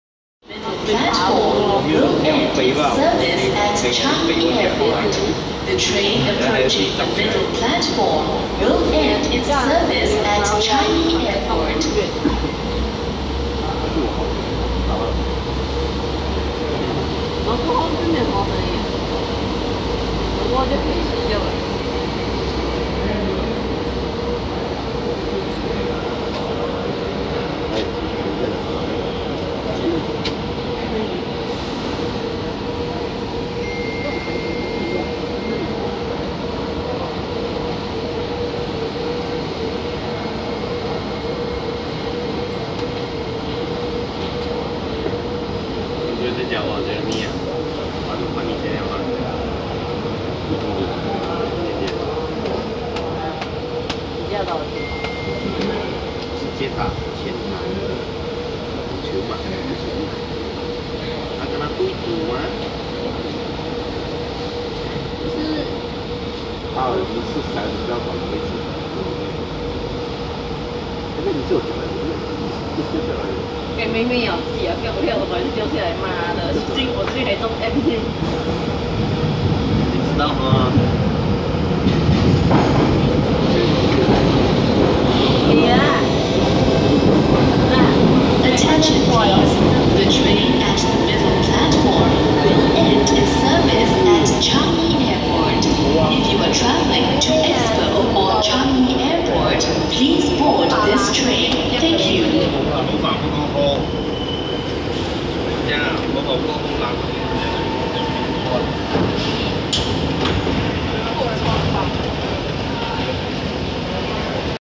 Exclusive SMRT C151 3030to3031 Changi Airport !!!
This video is temporary upload
Taken on: 11 November 2015
SMRT Train C151 Depart Changi Airport !
Time: 11:17